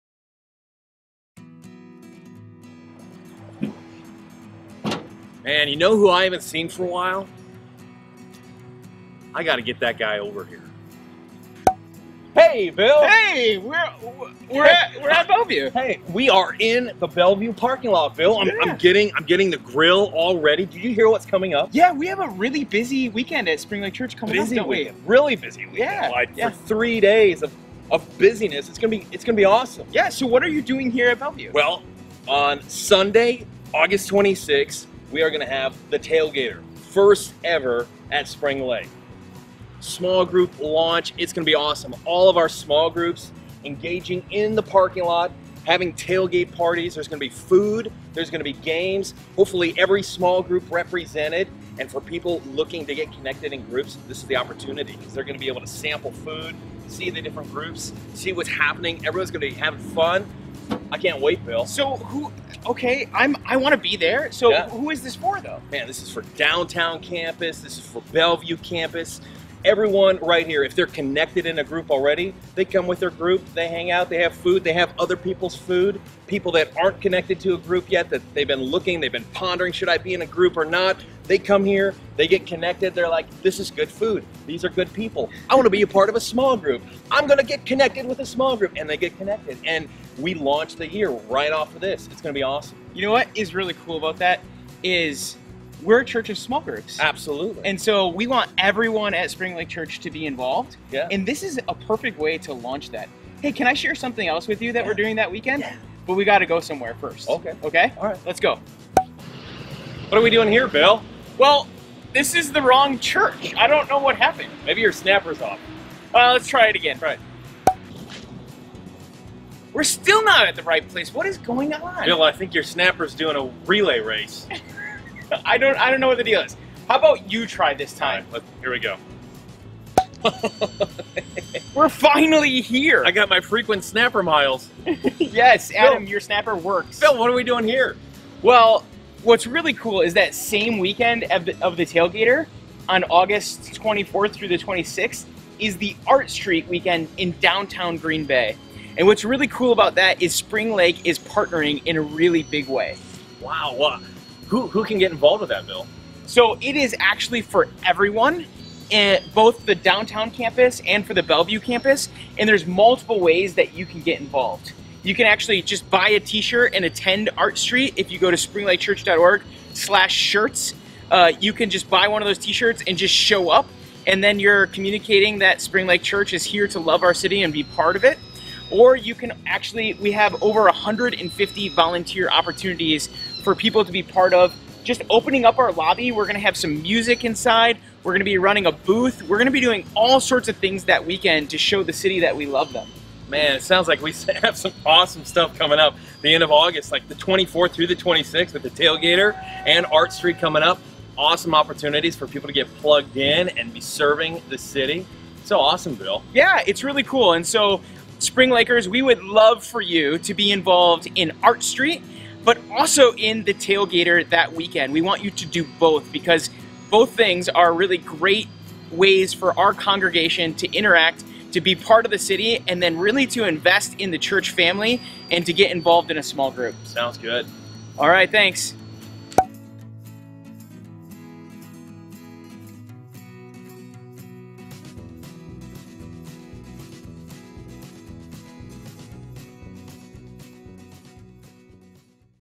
Artstreet Tailgater Aug 24-26
We have a big weekend coming up and we everyone to be involved! Aug 24-26th

Artstreet (Downtown)

- Pray for the event
- Attend the event
- There will be Downtown Service on Aug 26, but there will be live worship music.

Small Group Tailgater (Bellevue 6-8pm, Aug 26)
-Attend with your small group
-Find a small group if you don't have one.
-Lots of Food and Fun